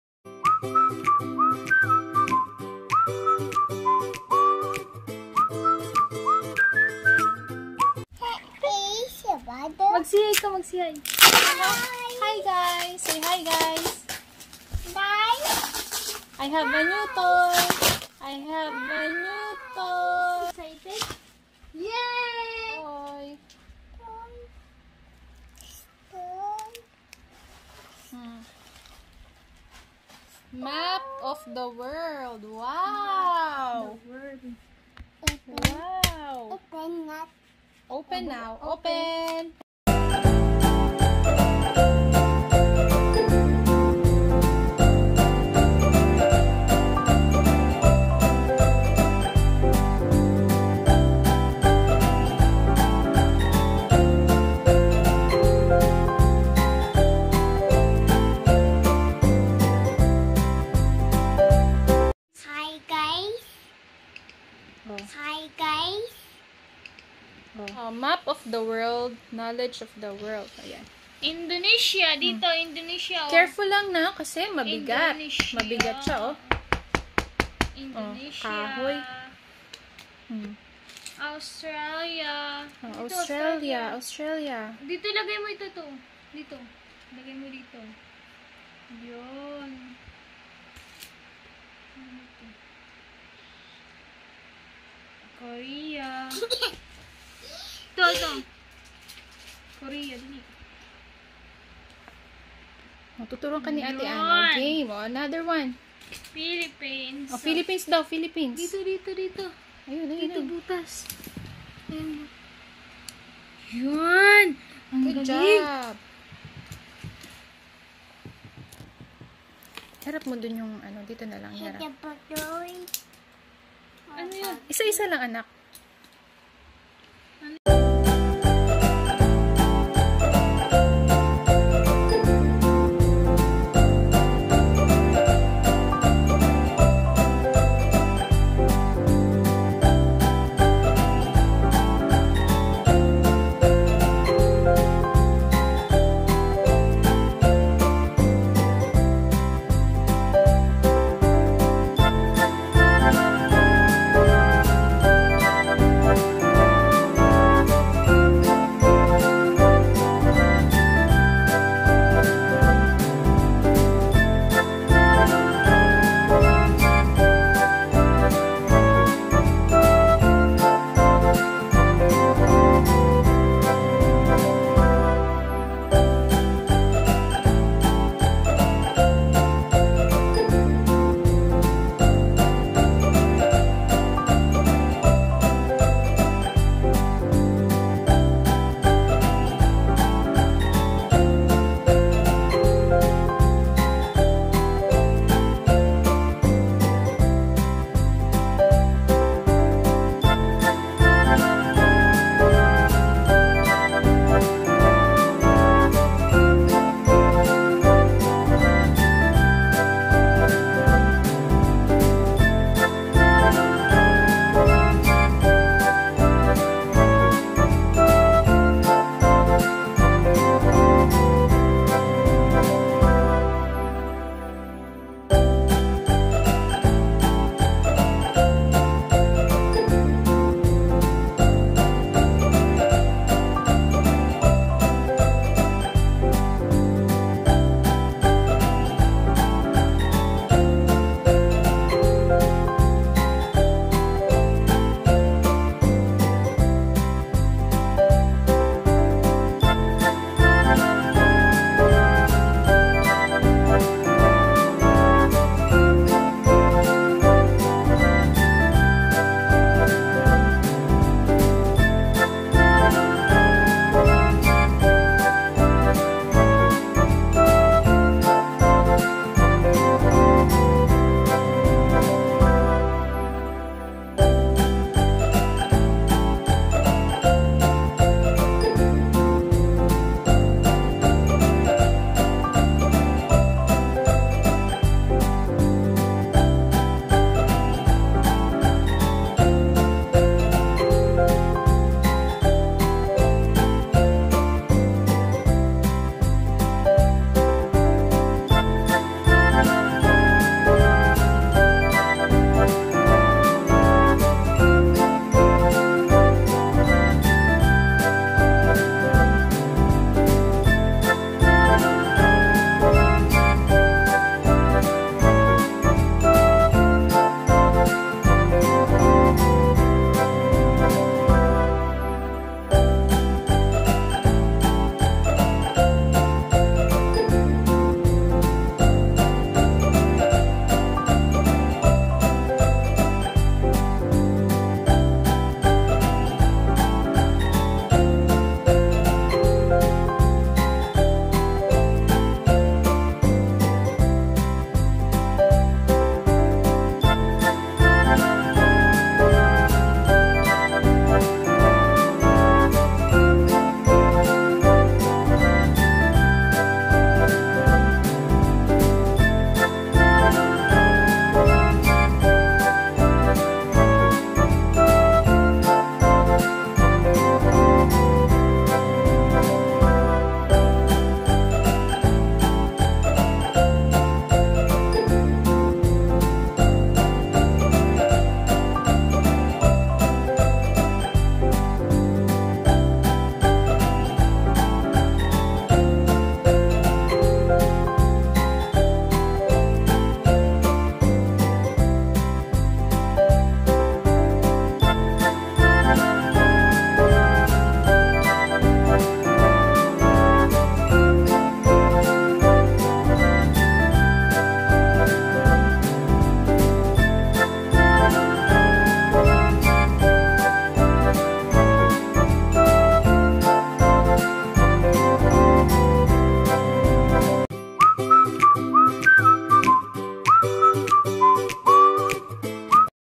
Learning about National Flags with Ate Ann
UNBOXING: Wooden Toy World Map with National Flags